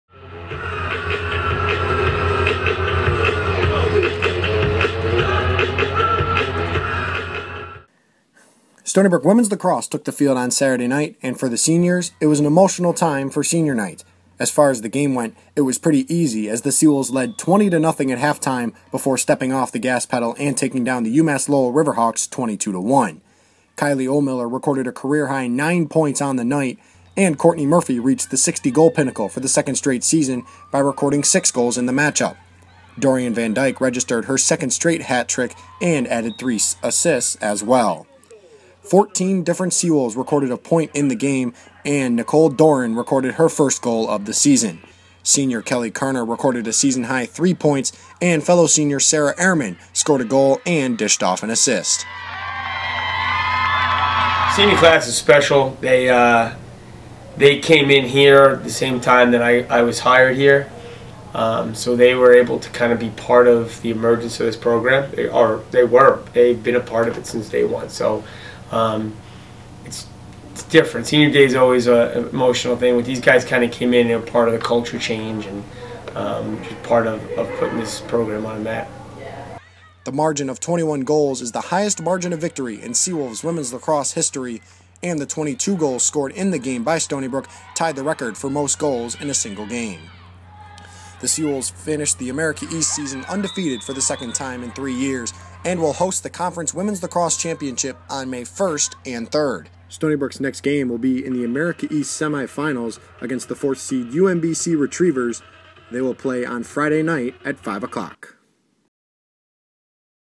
WLAX: SBU vs. UMass Lowell
Stony Brook women's lacrosse team defeated UMass Lowell at home, 22-1.